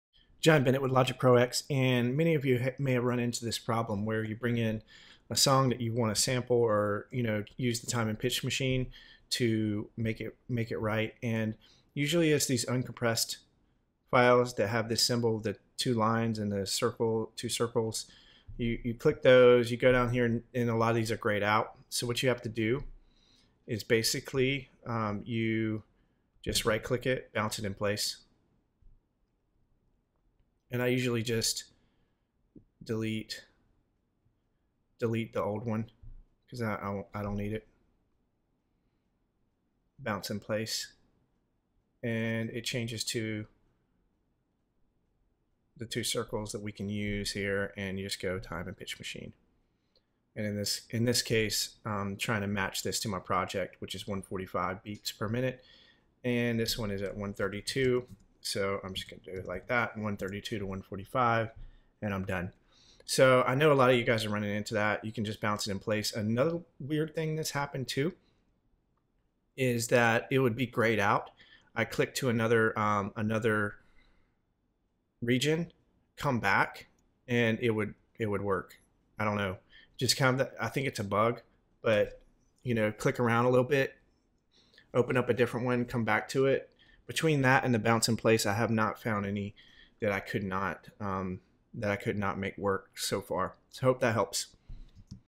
Logic Pro X Time and Pitch Machine Greyed Out
- Go here for more detailed tutorials - Free Signup

Many people are having trouble using the time and pitch machine as well as other tools on "compressed" files that have been imported.

I show you how to convert them by bouncing the file in place so that you can you all the available tools.